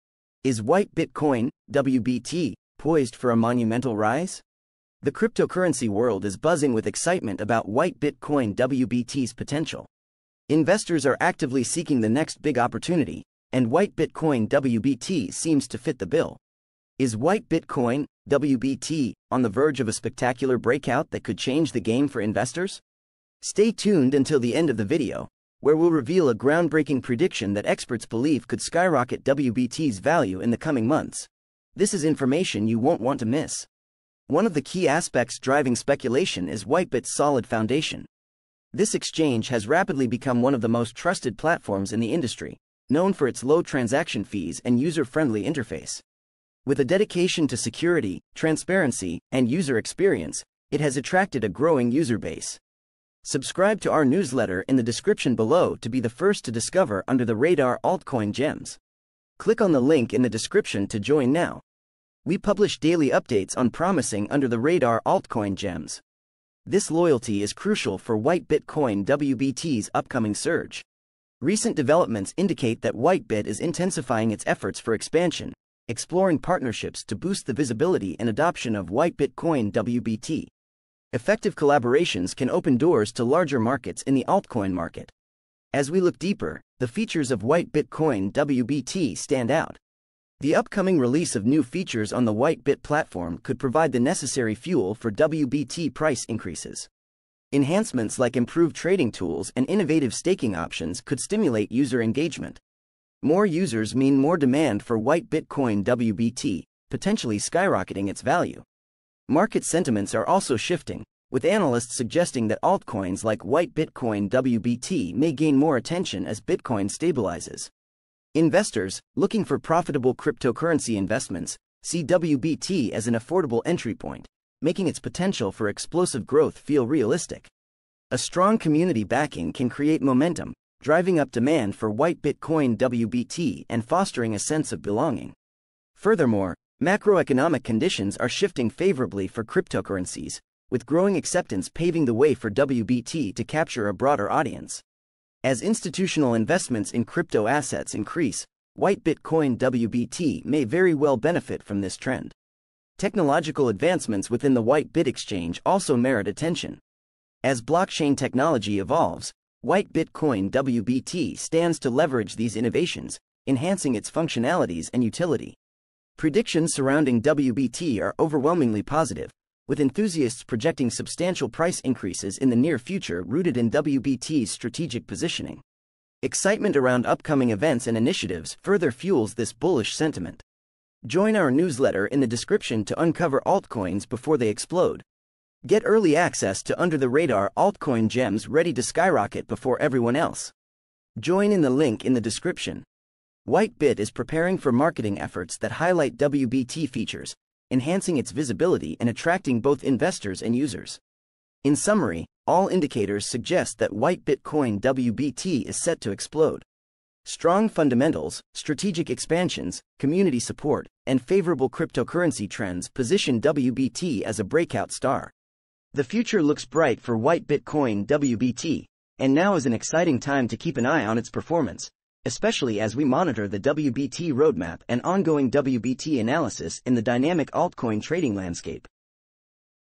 Is WhiteBIT Coin (WBT) Set to Explode? Uncover the Secrets of its Upcoming Surge!
Is WhiteBIT Coin (WBT) poised for a monumental rise? Discover the secrets of its upcoming surge in this video. We delve into the potential for explosive growth in WBT, driven by its solid foundation, strategic partnerships, user-friendly features, and market trends. With a strong community backing, favorable macroeconomic conditions, and technological advancements within the WhiteBIT ecosystem, WBT is positioned as a potential breakout star in the altcoin market.